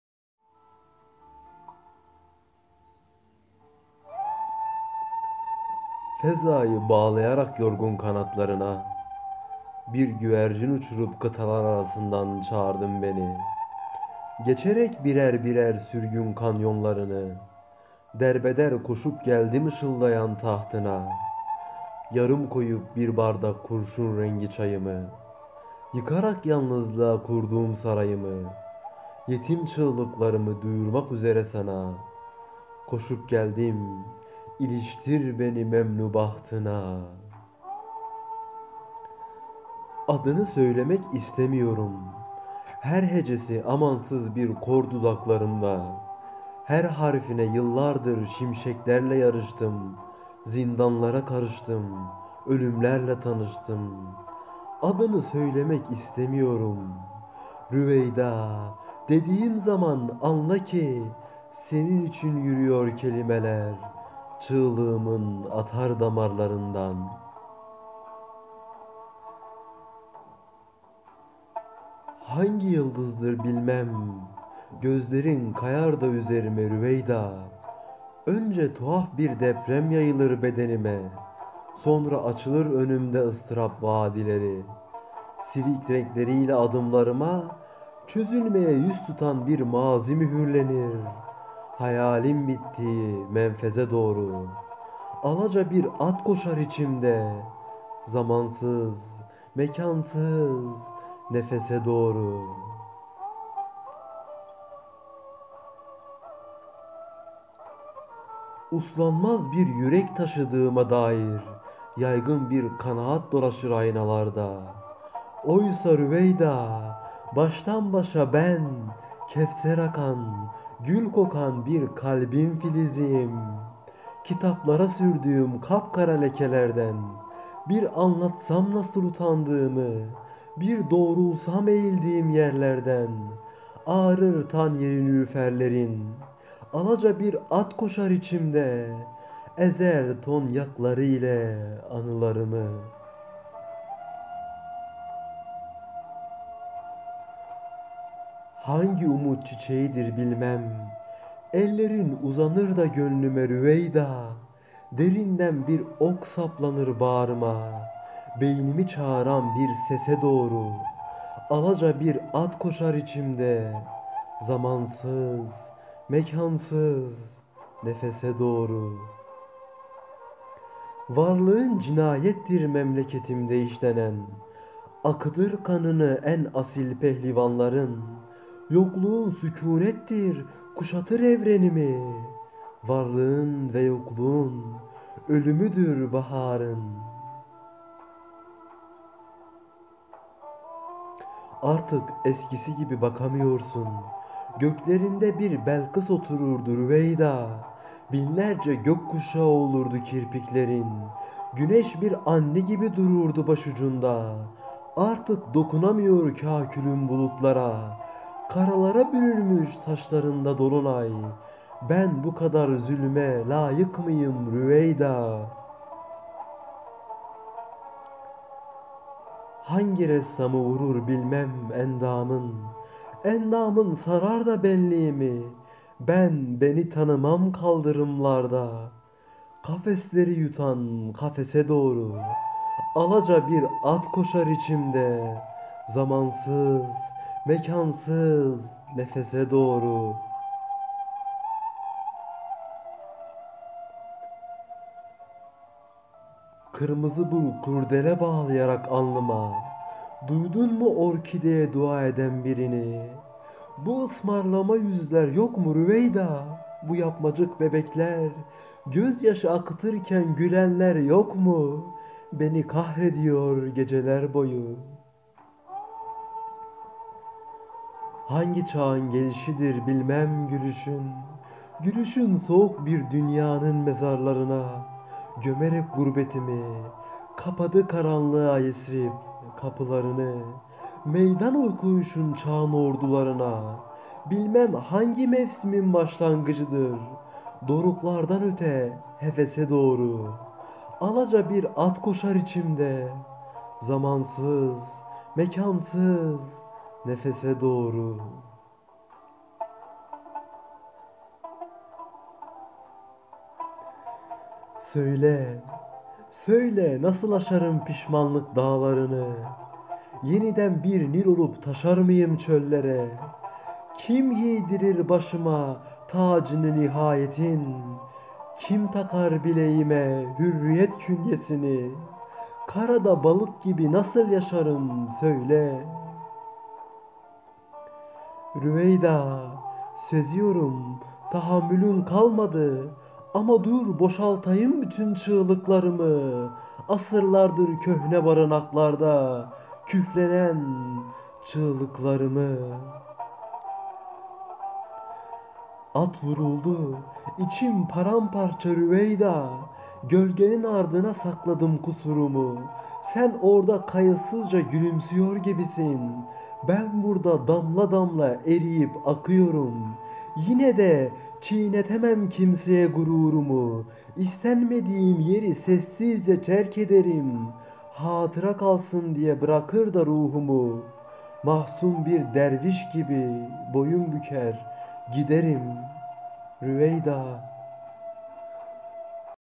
Rüveyda - Seslendirmeye çalışan: Mücahit TOPKAYA
fezayı bağlayarak yorgun kanatlarına
bir güvercin uçurup kıtalar arasından
çağırdın beni
geçerek birer birer sürgün kanyonlarını
derbeder koşup geldim ışıldayan tahtına
yarım koyup bir bardak kurşun rengi çayımı
yıkarak yalnızlığa kurduğum sarayımı
yetim çığlıklarımı duyurmak üzre sana
koşup geldim; iliştir beni memnu bahtına

adını söylemek istemiyorum
her hecesi amansız bir kor dudaklarımda
her harfine yıllardır şimşeklerle yarıştım
zindanlara karıştım, ölümlerle tanıştım
adını söylemek istemiyorum
rüveyda dediğim zaman
anla ki, senin için yürüyor kelimeler
çığlığımın atardamarlarından

hangi yıldızdır bilmem, gözlerin
kayar da üzerime rüveyda
önce tuhaf bir deprem yayılır bedenime
sonra açılır önümde ıstırab vadileri
silik renkleriyle adımlarıma
çözülmeye yüz tutan bir mazi mühürlenir
hayalin bittiği menfeze doğru
alaca bir at koşar içimde
zamansız, mekansız nefese doğru

uslanmaz bir yürek taşıdığıma dair
yaygın bir kanaat dolaşır aynalarda
oysa rüveyda
baştanbaşa ben
kevser akan, gül kokan bir kalbin filiziyim.

kitaplara sürdüğüm kapkara lekelerden
bir anlatsam nasıl utandığımı
bir doğrulsam eğildiğim yerlerden
ağarır tanyeri nilüferlerin
ezer toynakları ile anılarımı

sular köpürmemeliydi rüveyda
kırılmamalıydı ıslak dalları hasret selvilerinin
ben zehire alışkınım, şerbete değil
rüyalar hefret eder avare duruşumdan
kabuslar çeker ancak derdimi yeryüzünde
sen gün boyu simsiyah bir ufukla beraber
ben her gece bir Mehdi türküsüyle çilekeş
yargılamak için zeval kayıtlarını
inkılab bekliyorum

hangi umut çiçeğidir bilmem, ellerin
uzanır da gönlüme rüveyda
derinden bir ok saplanır bağrıma
beynimi çağıran bir sese doğru

varlığın cinayettir memleketimde işlenen
akıtır kanını en asil pehlivanların
yokluğun sükunettir kuşatır evrenimi
varlığın ve yokluğun ölümüdür baharın

artık eskisi gibi bakamıyorsun
göklerinde bir belkıs otururdu rüveyda
binlerce gökkuşağı olurdu kirpiklerin
güneş bir anne gibi dururdu başucunda
artık dokunamıyor kakülün bulutlara
karalara bürünmüş saçlarında dolunay
ben bu kadar zulme layık mıyım rüveyda

hangi ressamı vurur bilmem, endamın
sarar da benliğimi
ben beni tanımam kaldırımlarda
kafesleri yutan kafese doğru

kırmızı bir kurdela bağlayarak alnına
duydun mu orkideye dua eden birini
bu ısmarlama yüzler yok mu rüveyda
bu yapmacık bebekler
gözyaşı akıtırken gülenler yok mu
beni kahrediyor geceler boyu

hangi çağın gelişidir bilmem, gülüşün
soluk bir dünyanın mezarlarına
gömerek gurbetimi
kapadı karanlığa Yesrip, kapılarını
meydan okuyuşun çağın ordularına
bilmem hangi mevsimin başlangıcıdır
doruklardan öte hevese doğru

yasını tutuyorum kararttığım düşlerin
yıpranmış divaneler gibiyim sokaklarda
amansız bir ütopya üfleyen pencereler
lif lif yoluyor dram seyyahı bedenimi
önümde, haksızlığın hesaba çekildiği
hiç kimsenin kimseyi tanımadığı mahşer
arkamda, kare kare ömrümü belirleyen
hatırladıkça yanıp tutuştuğum resimler

söyle, nasıl aşarım pişmanlık dağlarını
yeniden bir nil olup taşar mıyım çöllere
kim giydirir başıma tacını nihayetin
kim takar bileğime hürriyet künyesini
karada balık gibi nasıl yaşarım, söyle

rüveyda, seziyorum; tahammülün kalmadı
ama dur, boşaltayım bütün çığlıklarımı
asırlardır köhne barınaklarda
küflenen, çürüyen çığlıklarımı

at vuruldu; içim paramparça rüveyda
gölgelerin ardına sakladım kusurumu
sen orda kayıtsızca gülümsüyor gibisin
ben burda damla damla eriyip akıyorum
yine de, çiğnetemem kimseye gururumu
istenmediğim yeri sessizce terkederim
hatıra kalsın diye bırakır da ruhumu
mahzun bir derviş gibi boyun büker, giderim

Nurullah Genç